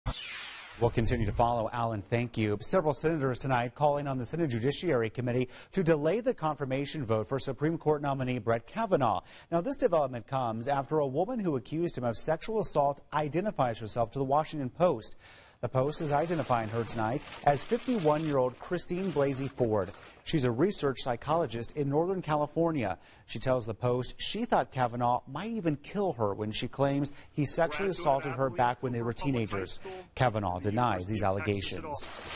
Kavanaugh Accuser Comes Forward
The woman who accused Kavanaugh of sexual assault comes forward publicly.